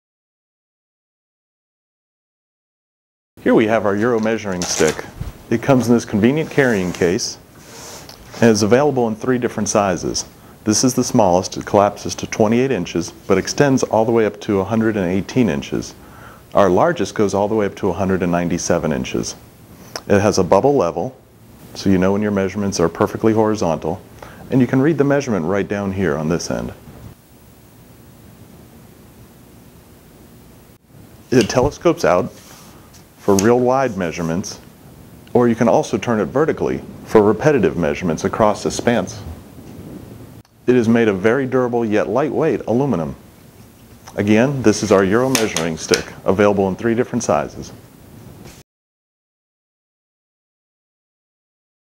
Euro Measuring Stick
Learn the features of our Euro Measuring Stick which telescopes up to 197", making it very easy to measure wide openings alone.  Made in Germany.